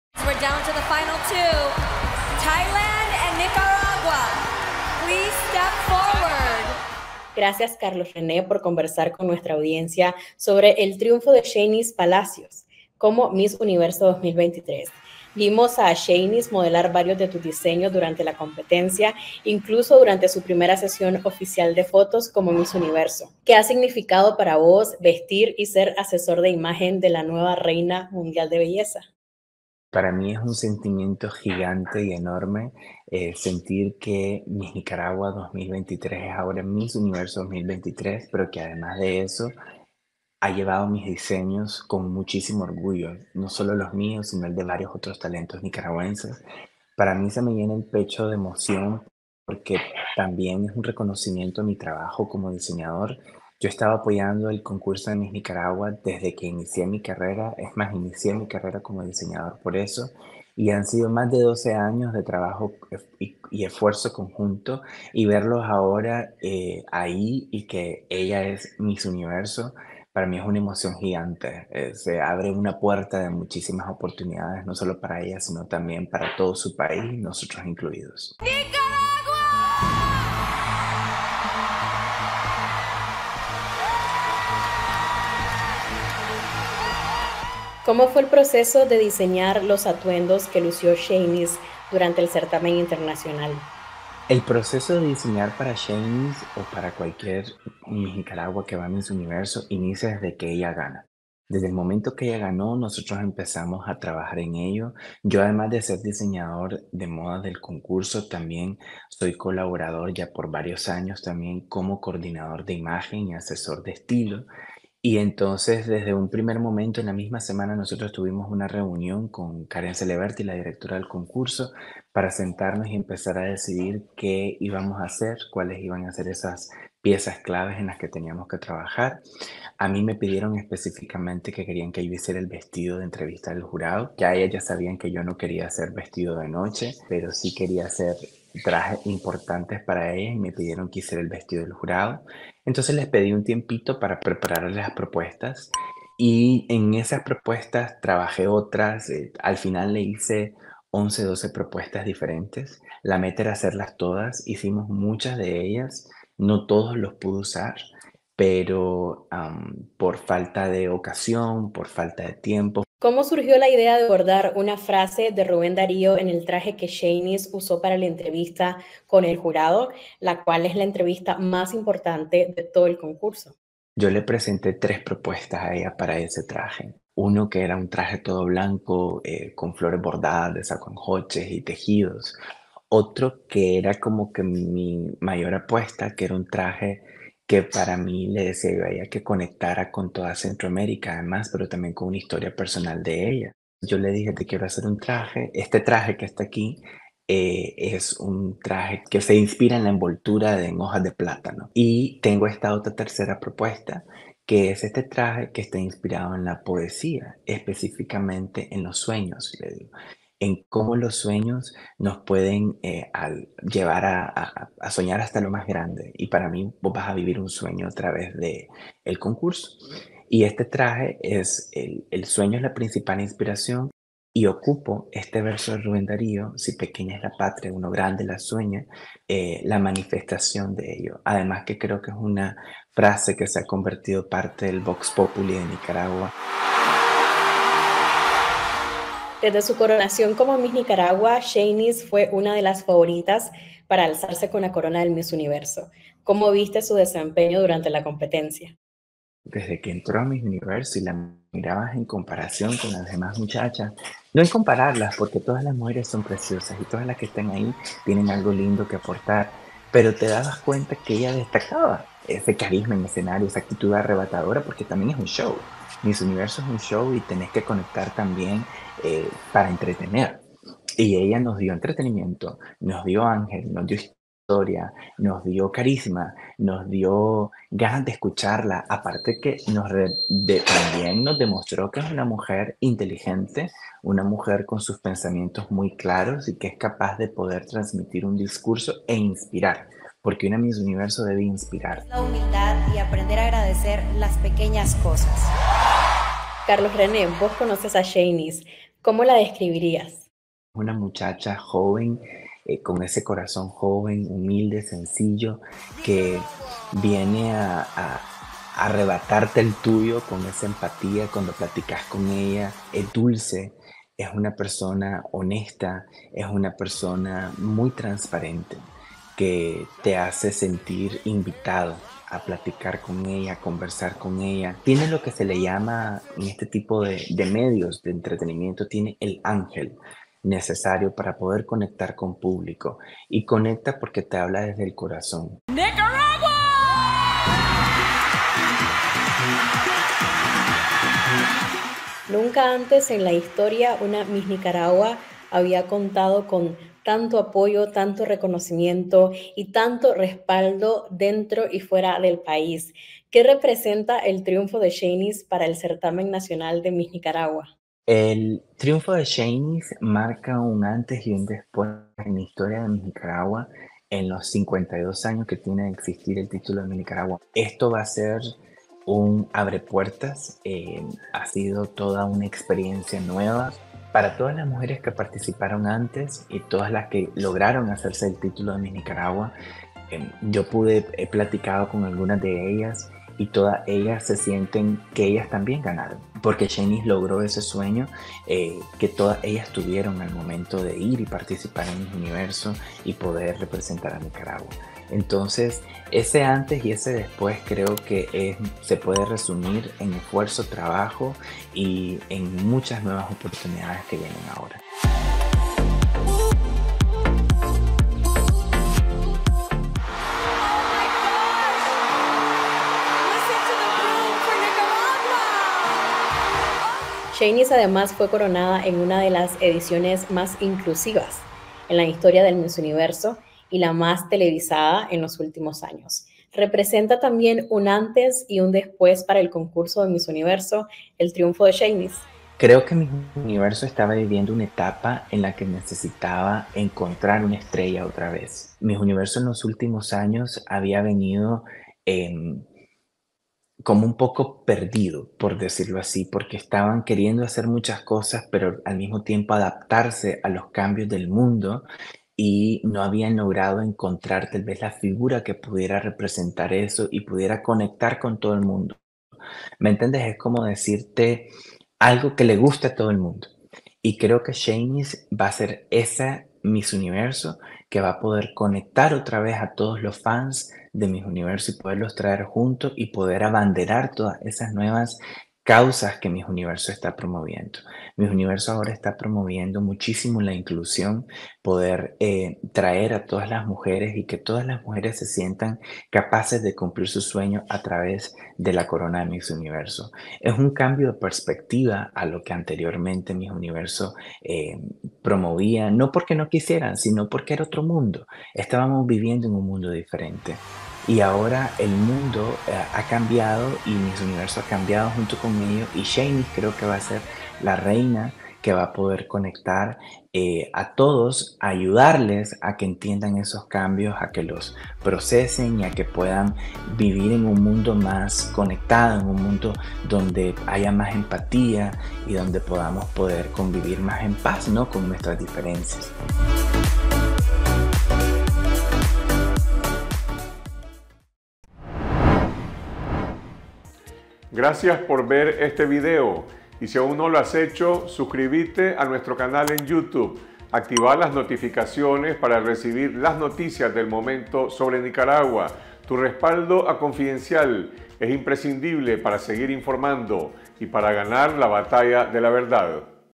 “Sheynnis Palacios es la reina que Miss Universo necesitaba”
El triunfo de la nicaragüense Sheynnis Palacios como Miss Universo 2023, marca un hito en el certamen de belleza internacional y en el Miss Nicaragua, porque se convirtió en la primera pinolera y centroamericana en alzarse con la corona universal, “abriendo las puertas” para que otras reinas de belleza de Nicaragua y la región aspiren a cumplir ese sueño, asegura Carlos René Cruz, diseñador nicaragüense y asesor de imagen de Miss Nicaragua, en esta entrevista.

“Sheynnis tiene un ángel. Es una muchacha humilde y con un gran corazón”, agrega Cruz, quien confeccionó varios de los atuendos que Palacios usó durante la competencia de Miss Universo, realizada en El Salvador, el 18 de noviembre de 2023.

Conoce cuáles son las cinco Miss Nicaragua que han hecho historia en los “top” de Miss Universo, AQUÍ en nuestro sitio web 👉🏻